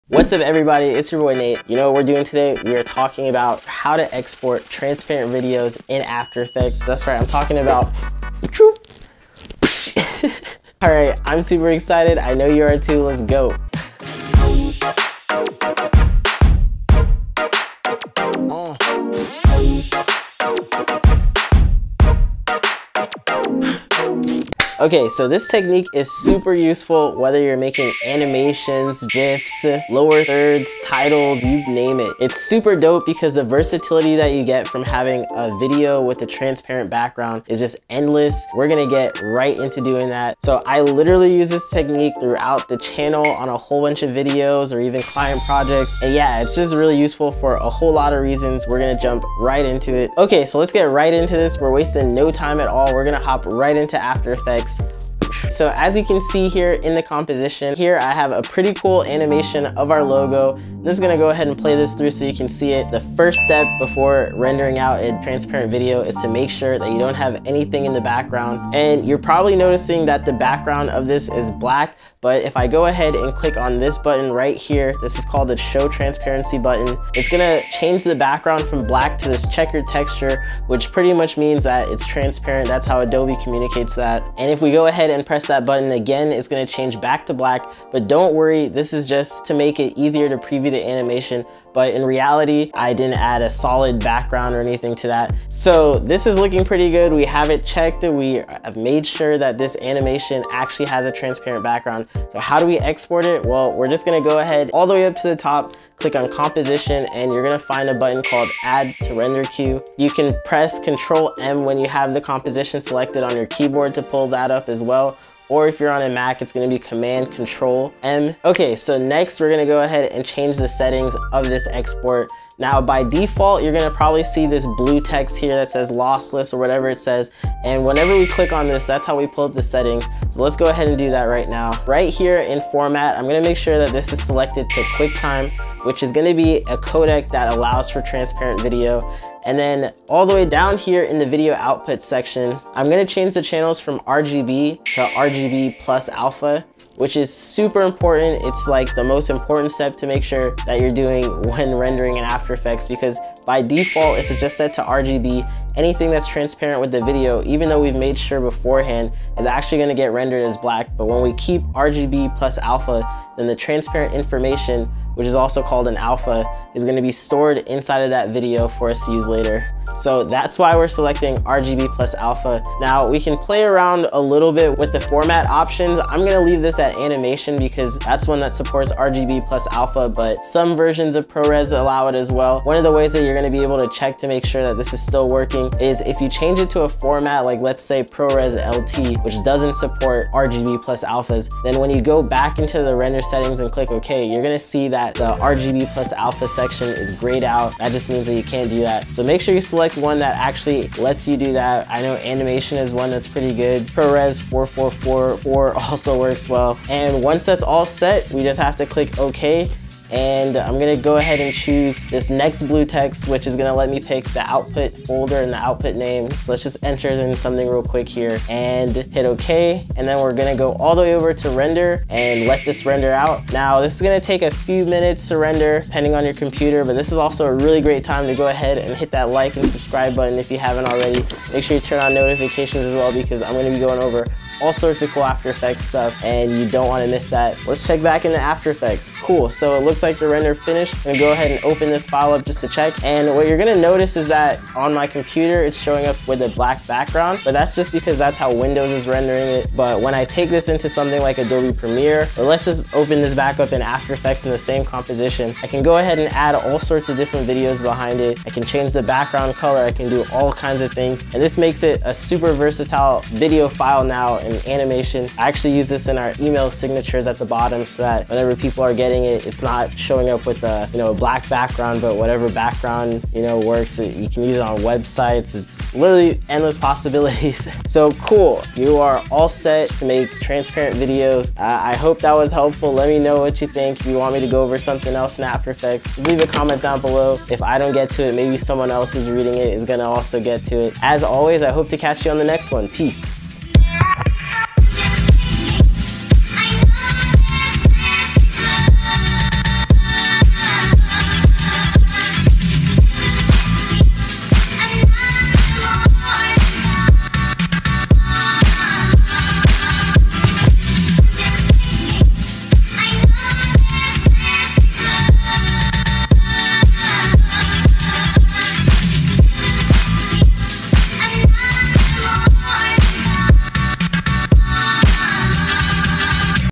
How to Export Transparent Background Videos in After Effects
Okay, today we are going over a super useful technique in After Effects! Have you ever wanted to make lower thirds, animations, gifs, or VFX shots that need a transparent background? Well even if you haven't, I guarantee you will need to at some point when using Adobe After Effects to make cool visuals. There's a bunch of uses for transparent videos and in my 10 years experience as a motion designer and VFX artist, I have used transparent video countless times throughout my projects. Stick around because we are diving right into exporting and rendering transparent video with After Effects.

SUGGESTED VIEWING:
AFTER EFFECTS BASICS VFX TUTORIAL 101